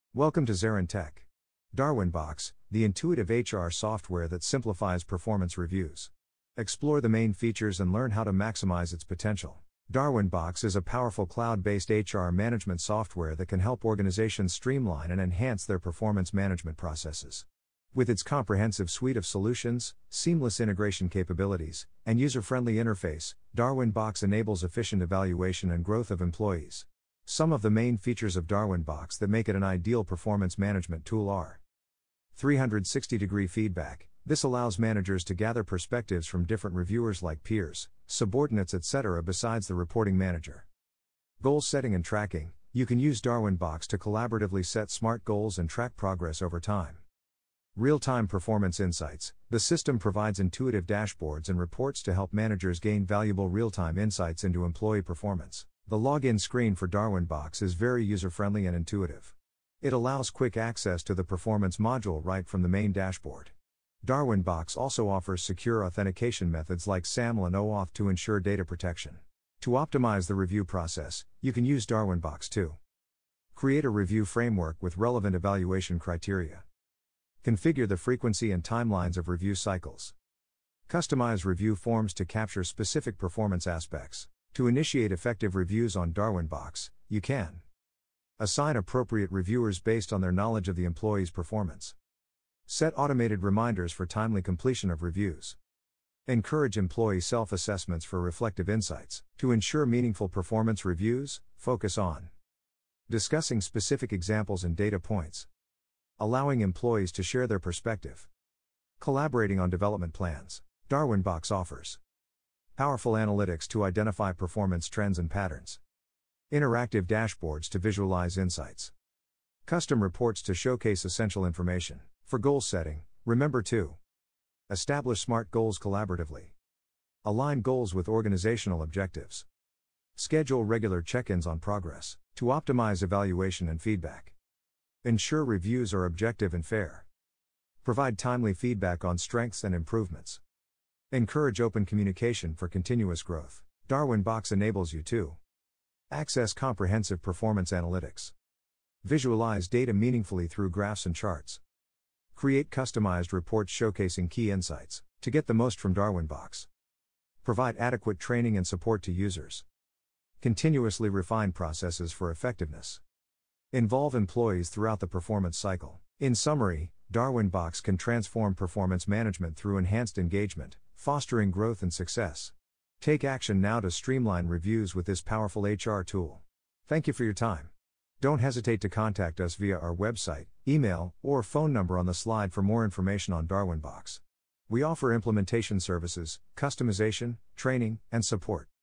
How to use Darwinbox to manage performance reviews | ZaranTech
👉 In this video you will learn about The future of data management and how DBT Cloud is leading the way.